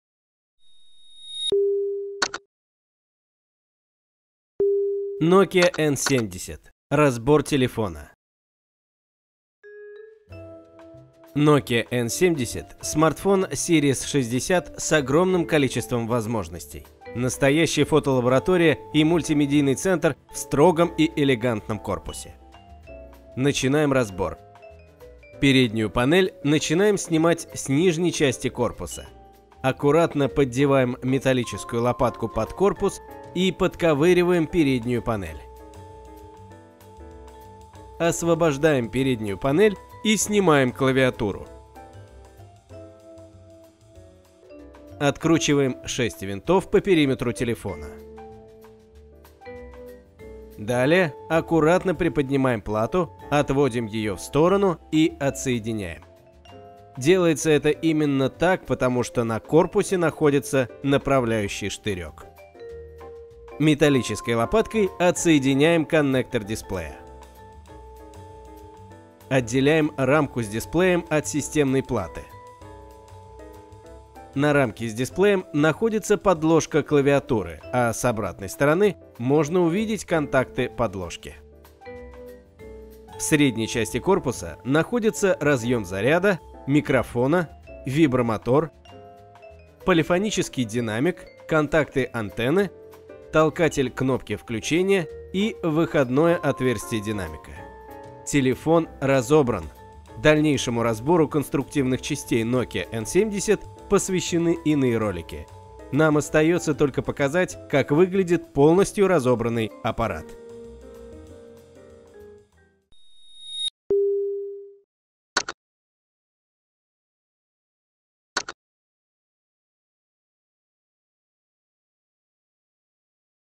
Nokia N70 - как разобрать телефон и из чего он состоит
Подробное пошаговое описание разбора мобильного телефона Нокиа н70: как разобрать телефон своими руками и произвести ремонт.